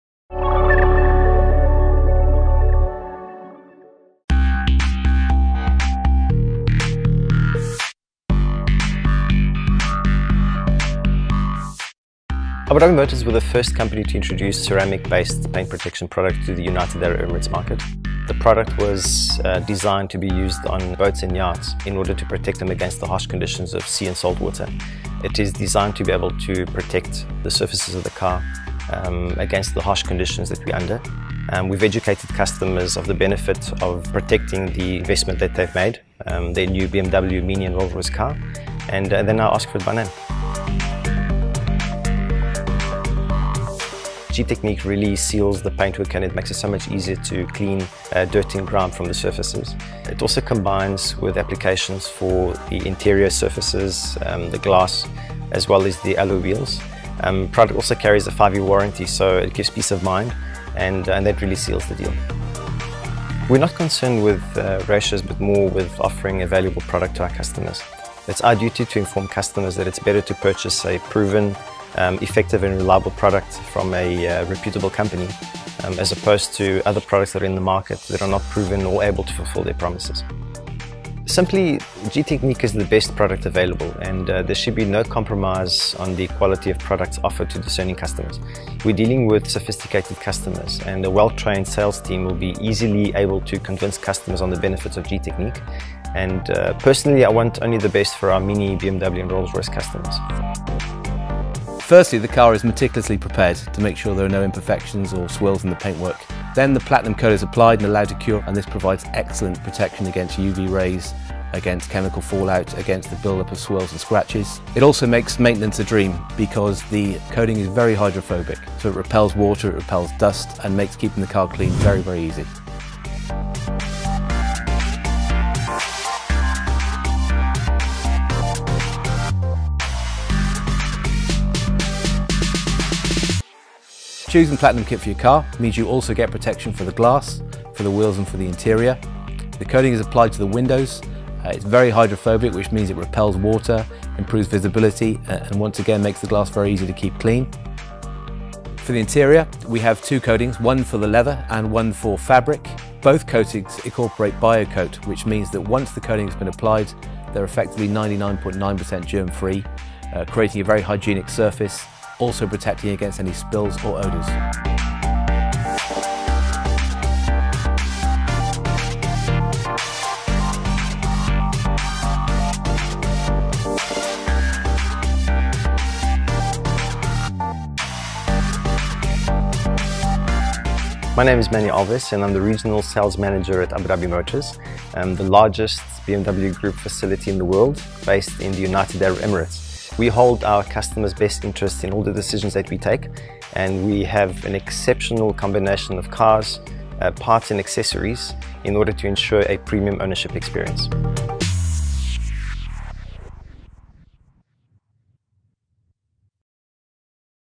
BMW Powłoka Ceramiczna Gtechniq Platinum / PL napisy
Gtechniq Platinum to system zabezpieczania nowych pojazdów, dostępny wyłącznie w salonach dealerskich. Usługa ma postać serii zabiegów, wykonanych przy użyciu preparatów Platinum opartych na najnowszych osiągnieciach nanotechnologii. Jej skuteczność przekracza jakiekolwiek inne rozwiązania dotychczas dostępne w salonach dealerskich.